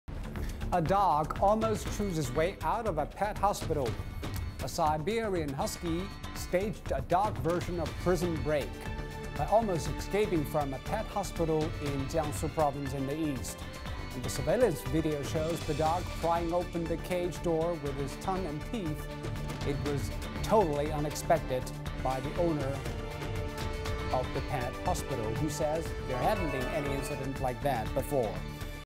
‘Prison Break’ dog makes run for freedom
A Siberian Husky recently staged a canine version of "Prison Break" by almost escaping from a pet hospital in east China's Jiangsu Province. Surveillance video shows the dog prying open its cage door with its tongue and teeth, before exploring the hospital.